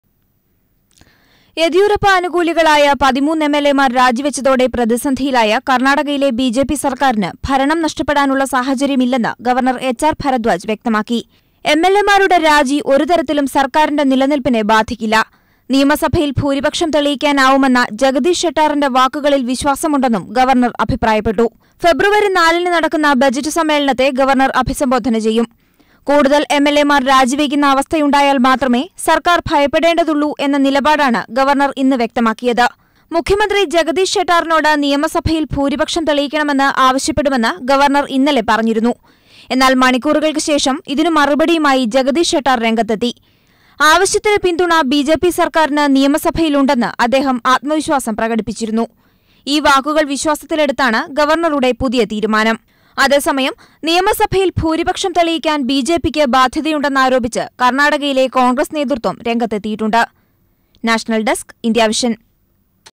No threat to Karnataka Government; Says Governor H R Bhardwaj
കര്‍ണാടകയില്‍ ജഗദീഷ് ഷെട്ടാര്‍ മന്ത്രിസഭയ്ക്ക് ഭീഷണിയില്ലെന്ന് ഗവര്‍ണര്‍ എച്ച.ആര്‍ ഭരദ്വാജ്. നിയമസഭയില്‍ വിശ്വാസവോട്ട് തേടാന്‍ മുഖ്യമന്ത്രിയോട് ആവശ്യപ്പെടില്ലെന്നും ഗവര്‍ണര്‍ അറിയിച്ചു.